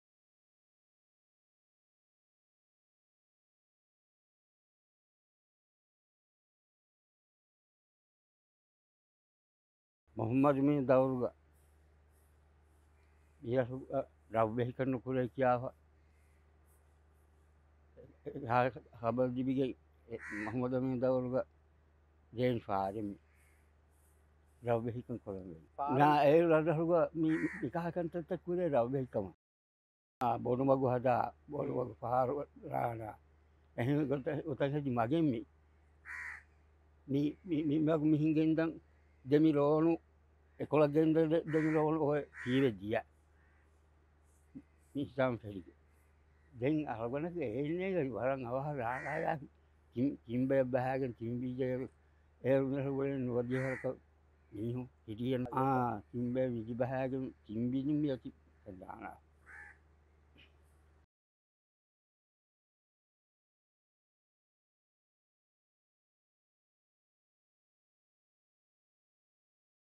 Coral stone wall
This wall was built during the presidency of Mohamed Amin Didi (January-August 1953) as a symbolic project to promote the development of the island. The entire population of the island (about 90 people) participated in the construction. They were divided into three groups, each of which built an equal length. Today, only part of the wall remains. The rest of the wall has been demolished or has become incorporated into peoples’ private property.

MHS Site No                  HAF-TKD-3
Atoll                                 Haa Alif
Island                             Thakandhoo